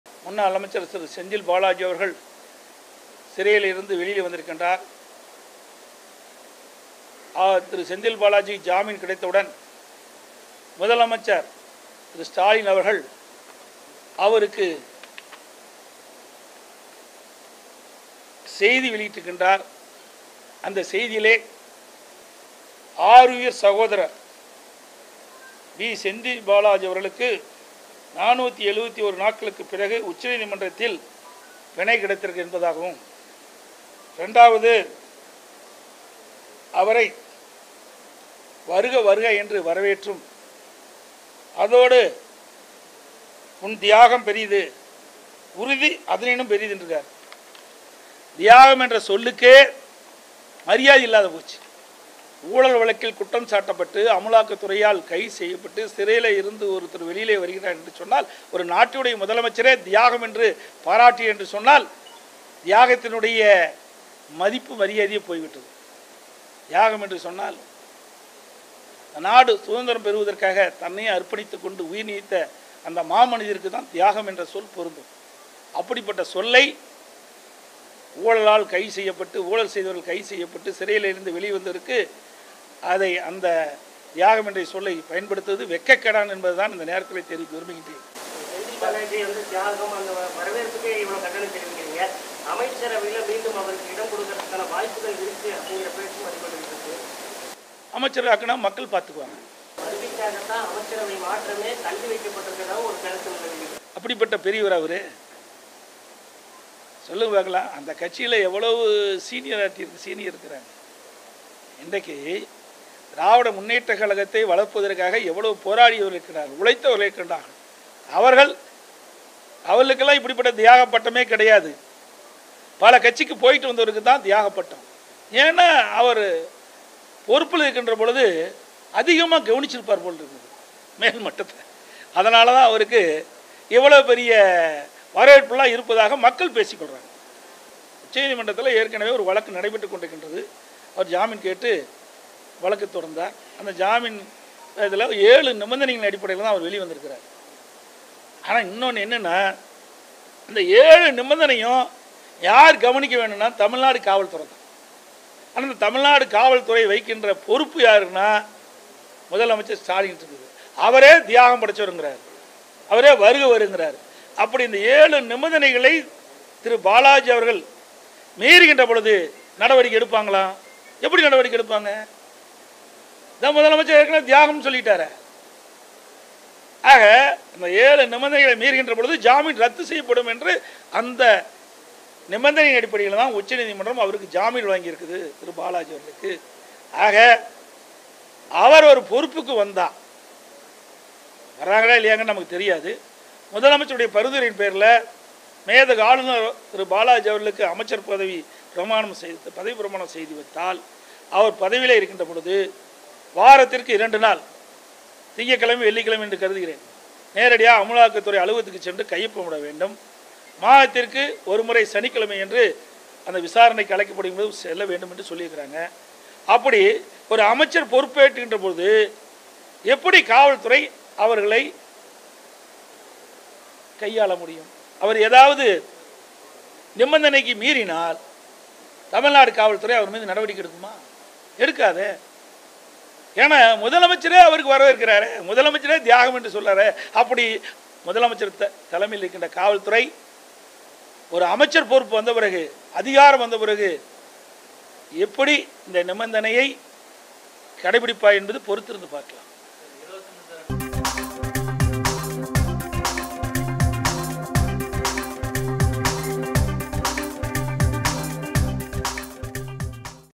அப்படி பட்ட பெரியவரா அவர்?..சிறையில் இருந்து வந்த செந்தில் பாலாஜி..முதல்வர் போட்ட பதிவு..EPS விளாசல்
அப்படி பட்ட பெரியவரா அவர்?.. சிறையில் இருந்து வெளியே வந்த செந்தில் பாலாஜி.. முதல்வர் போட்ட பதிவு.. தியாகம் என்ற சொல்லுக்கே மரியாதை இல்லாம போயிடுச்சு.. அமைச்சரவை மாற்றமா? - இ.பி.எஸ் விளாசல்